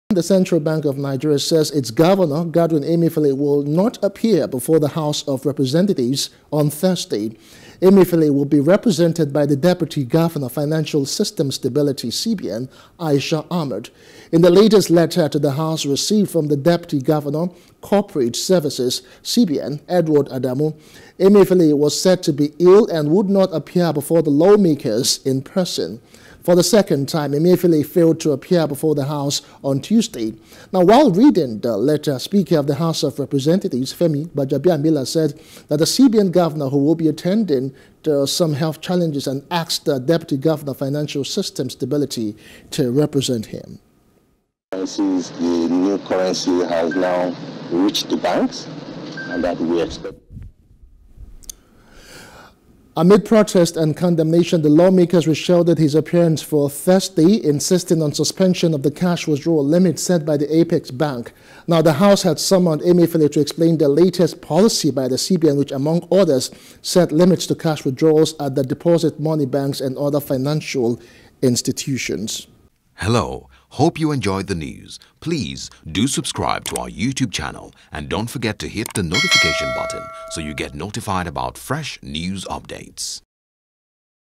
Cash Withdrawal Policy: C.B.N. Writes Reps. Over Emefiele’s Absence
The Central Bank of Nigeria says its Governor, Godwin Emefiele, will not appear before the House of Representatives on Thursday.

Emefiele will be represented by the Deputy Governor, Financial System Stability, CBN, Aisha Ahmad.